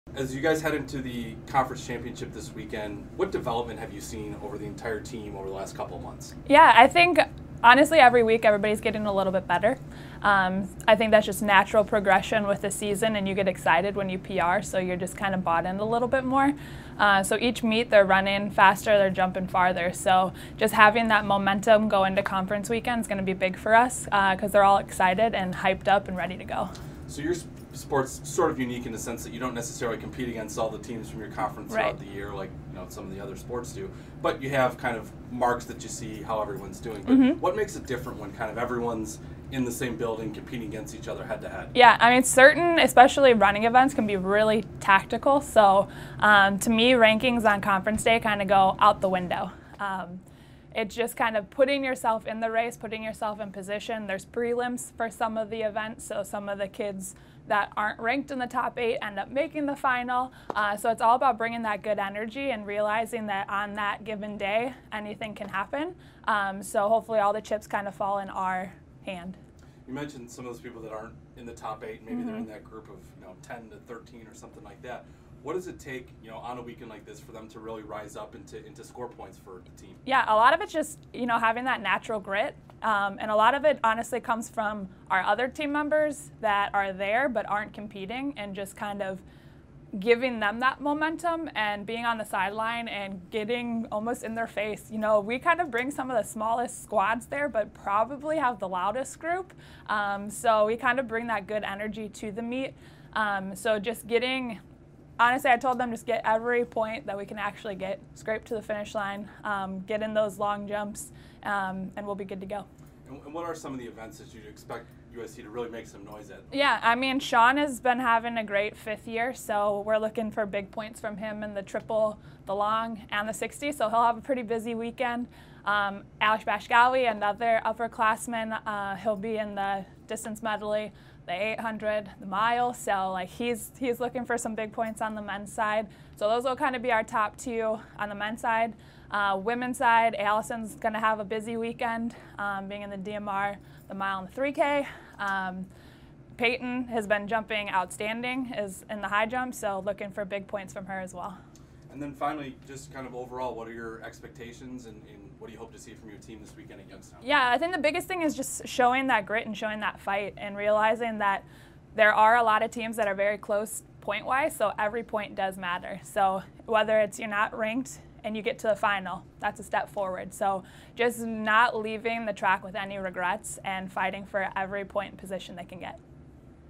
Horizon League Indoor Track & Field Championship Preview
UIC head track and field coach Ashley Lucas sat down with UICFlames.com to preview the conference championship at Youngstown State.